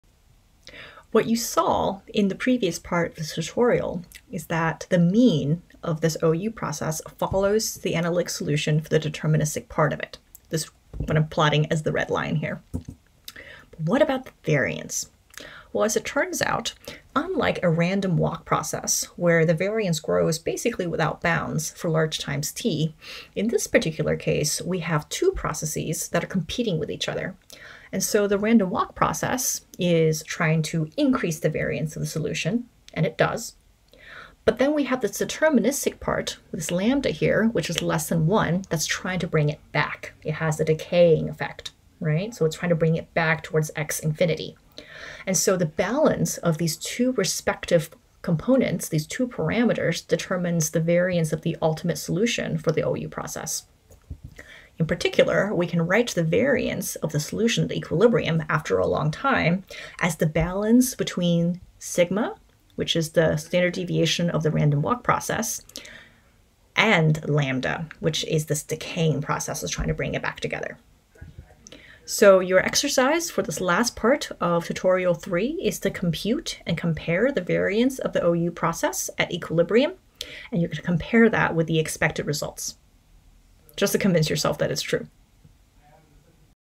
Linear Systems Tutorial 3: Part 3
Description: An exploration of the behavior of an Ornstein-Uhlenbeck (OU) process by examining its mean and variance at equilibrium.

We thank Tian Season Qiu for editing this video, and Madhuvanthi Muliya for manually editing the English captions.